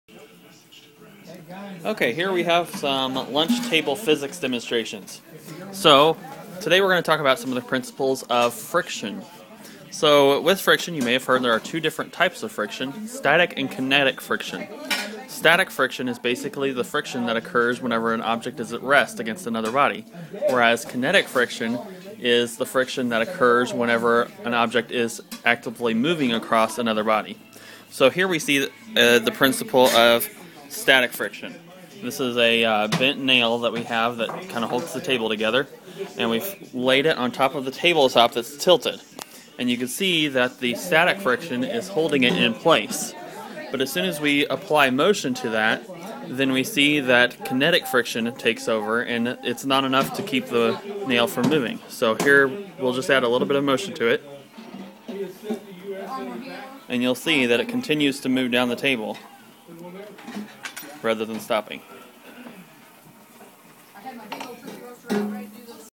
Lunch Table Science: Static Friction vs Kinetic Friction
A quick physics demonstration with everyday objects